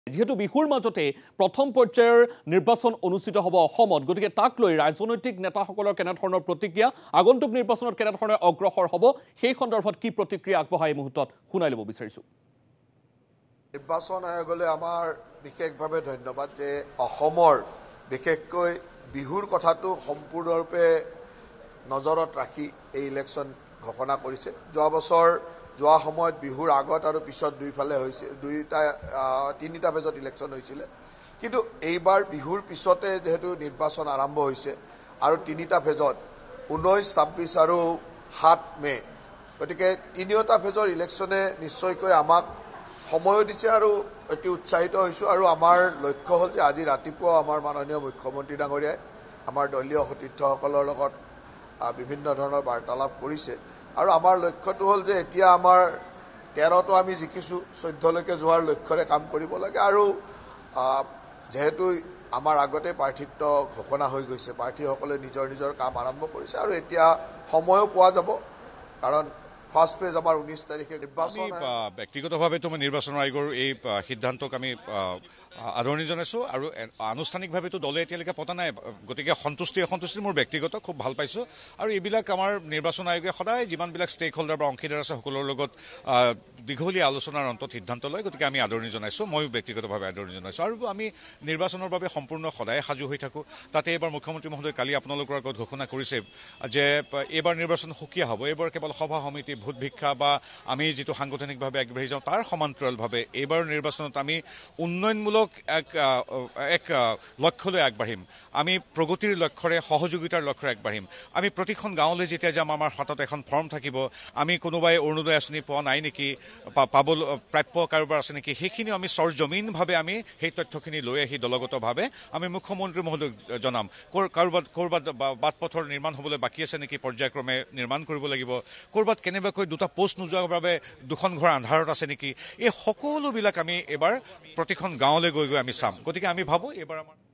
বিহুৰ মাজতে নিৰ্বাচন অনুষ্ঠিত কৰাক লৈ ৰাজনৈতিক নেতাসকলৰ কেনেধৰণৰ প্ৰতিক্ৰিয়া? election
বিহুৰ মাজতে নিৰ্বাচন অনুষ্ঠিত কৰাক লৈ ৰাজনৈতিক নেতাসকলৰ কেনেধৰণৰ প্ৰতিক্ৰিয়া?